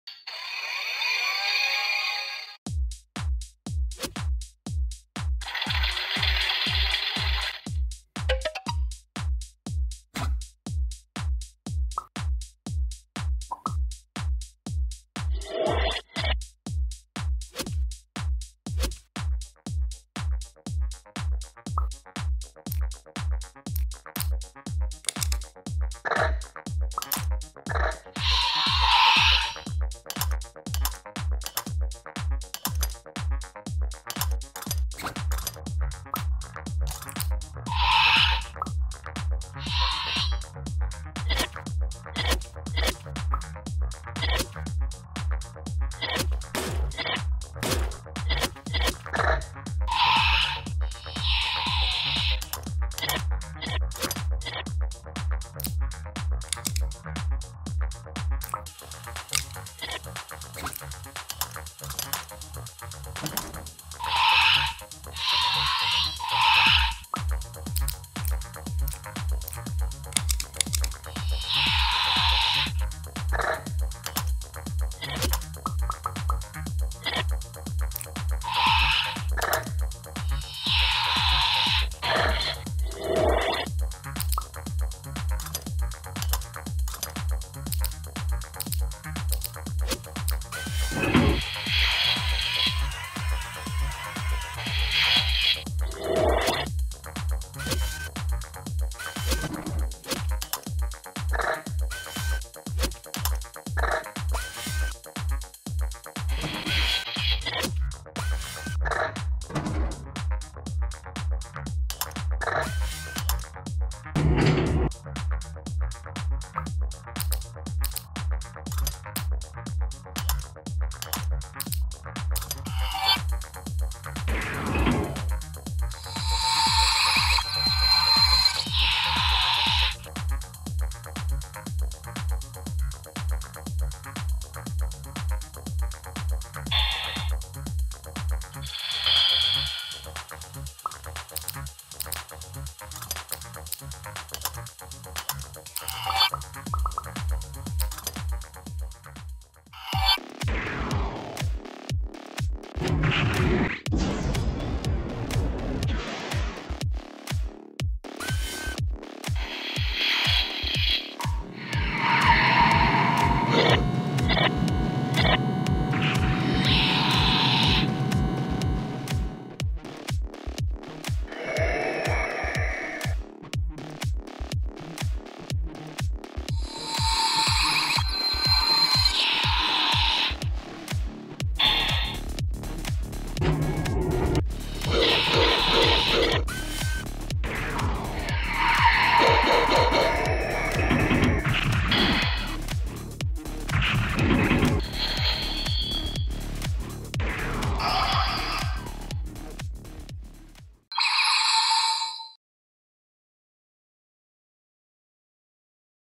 Hg Moon Gundam Stop Motion Build (Remake) HGUC 1/144 ムーンガンダム
ガンプラ ストップモーション

This channel make videos about Dx tokusatsu , gundam stop motion